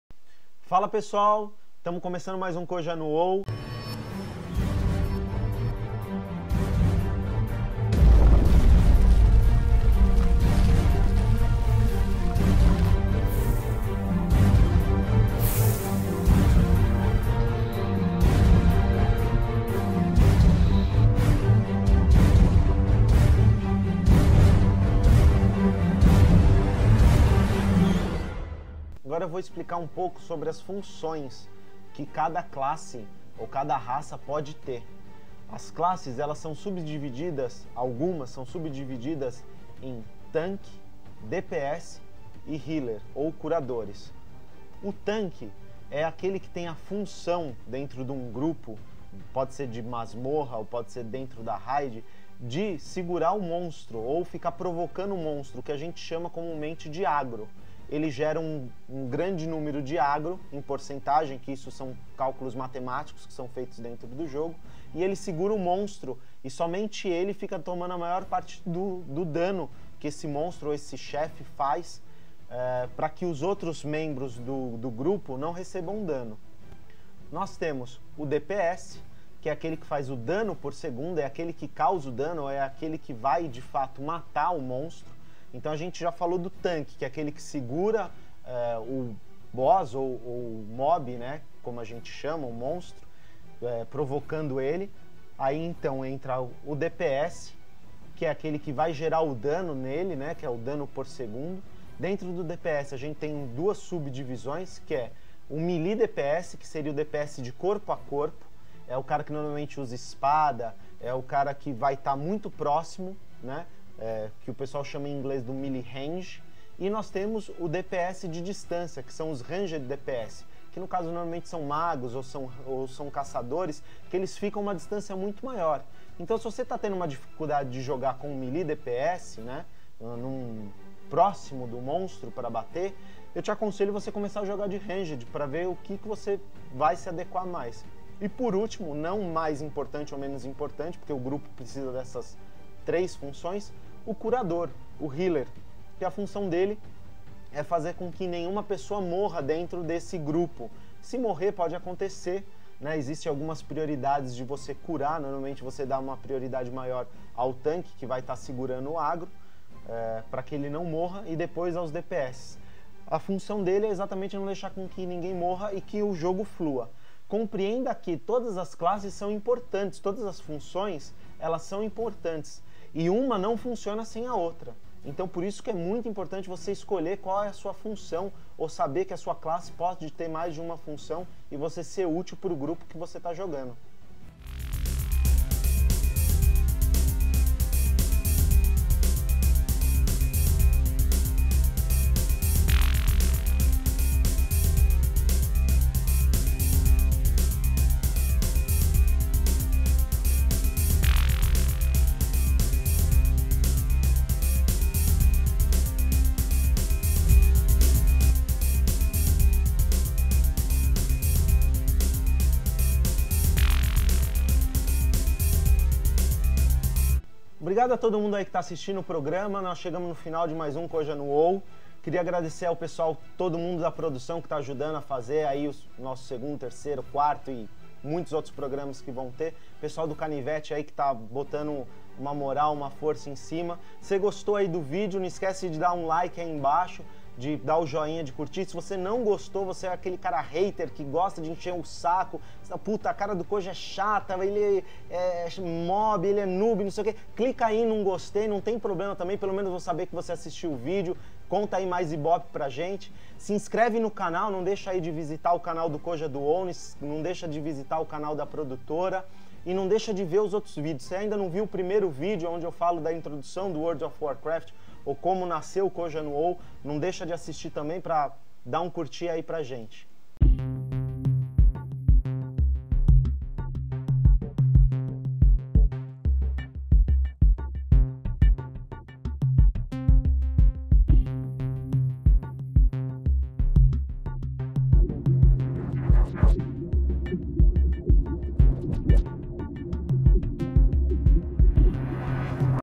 Coja no Wow - Programa 02 - Funções dentro do Jogo!!!
Segundo Programa Coja no Wow, Episodio 02 -Funções dentro do Jogo!!!.
O programa e o Site falam sobre os Achievement Points ou Pontos de Conquistas do Wow e conteúdo da Blizzard.
Toda a primeira terça-feira do mês um novo programa estará disponível.

PROGRAMA:
00:03 Abertura.
00:28 Início do Programa.
00:44 Tank.
01:15 DPS - Dano Por Segundo.
02:18 Healers ou Curadores
03:04 Wow de A á Zerg.
Palavras: LDT / Nome do Mundo do Wow / Fórum Oficial
03:37 Agradecimentos.
04:30 Créditos da Produção.